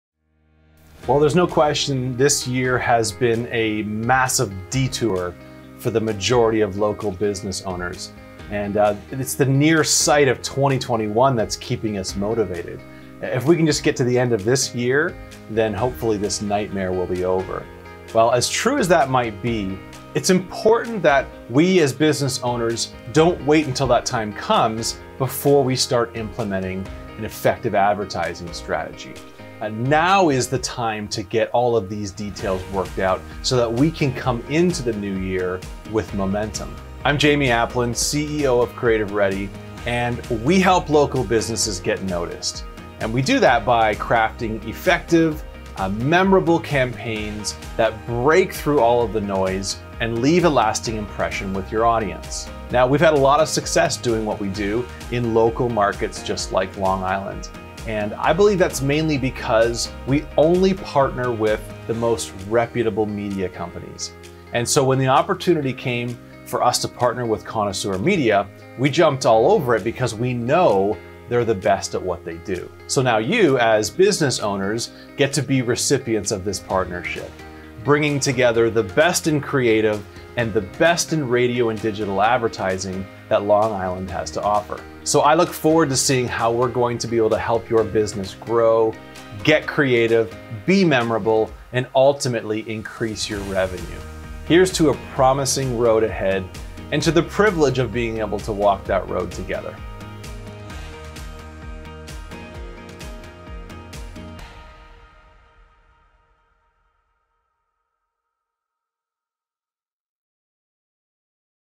Spotlight Long Island
A 90 second intro video for the Spotlight Long Island seminar.